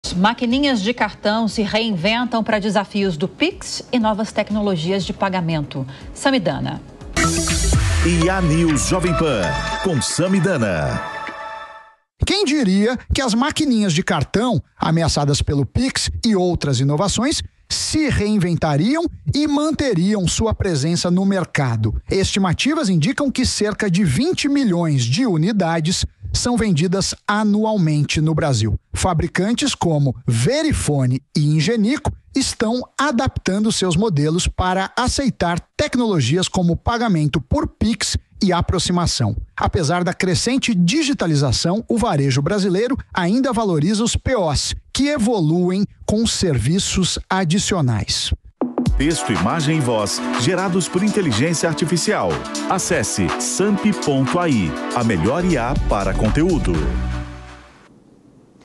IA News: Maquininhas de cartão se reinventam para desafios do Pix; Samy Dana detalha
As maquininhas de cartão se reinventam para os desafios do Pix. O IA News desta terça-feira (15) com Samy Dana detalhou mais sobre o assunto.